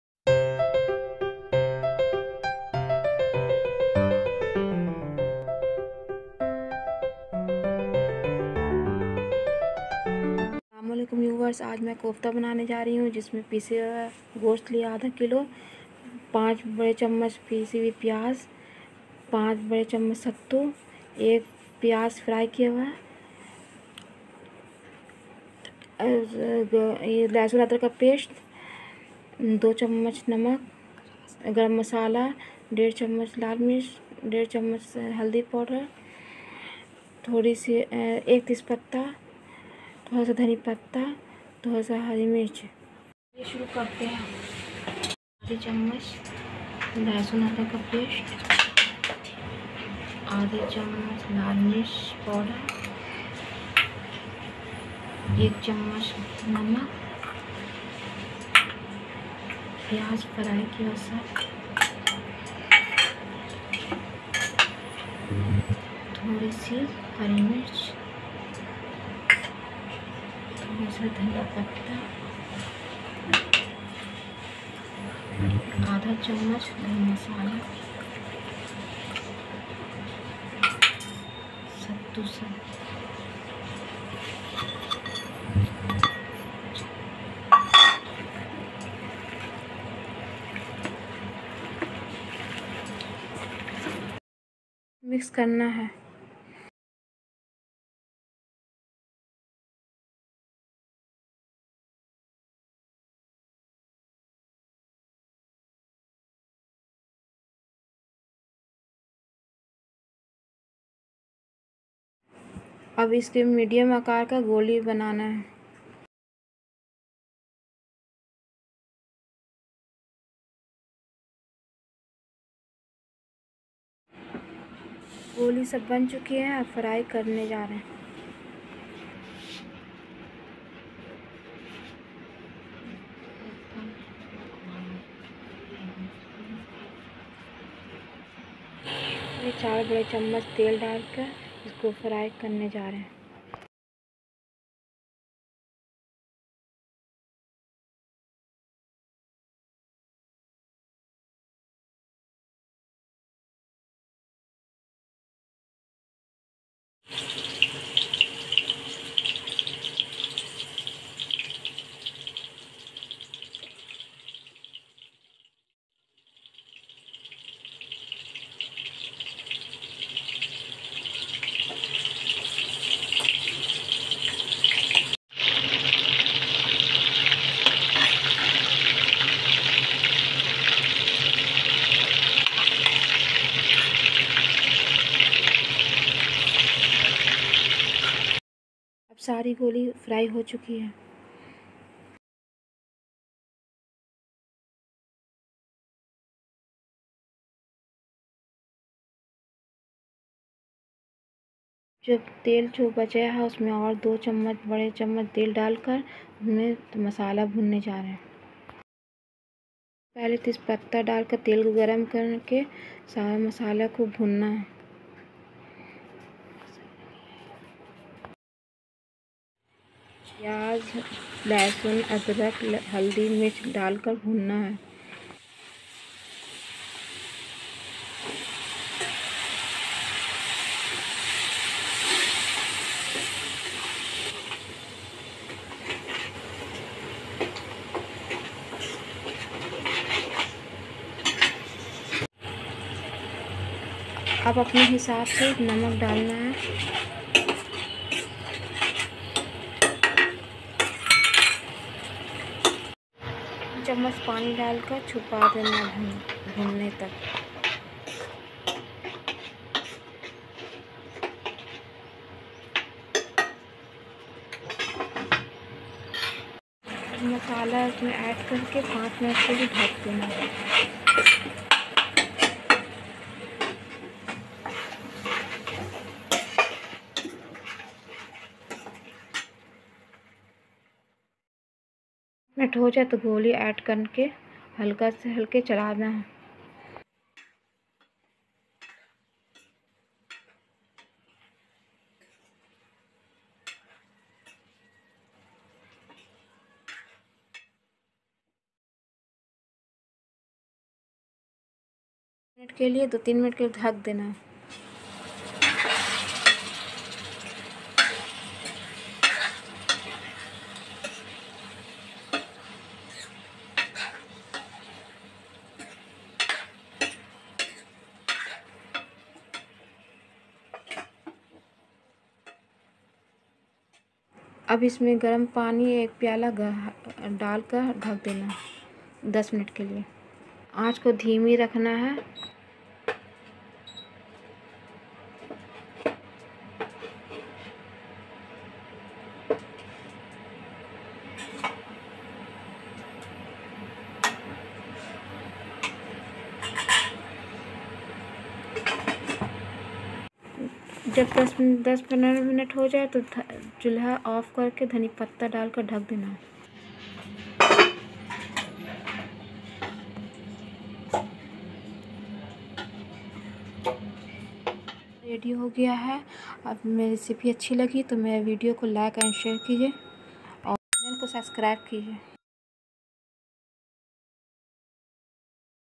Tasty😋 kofta
At 0:33 &0:35, I committed a little mistake in place of 1&1/2,I wrote 1/2.so please ignore it.
Ingredients needed:
a. 1/2 kg grind meat
b.5 spoon onion paste
c.5 spoon sattu
d.1 fried onion
e.2 spoon garlic,ginger paste
f.2 spoon salt(or as per taste)
g.1 spoon garam masala
h.1&1/2 spoon lal mirch powder
i.1&1/2 spoon turmeric powder
j.1 tej patta
k. few coriander leaves
l.4-5 green chillis
m.6 big spoon(doi) oil
n. 1 cup warm water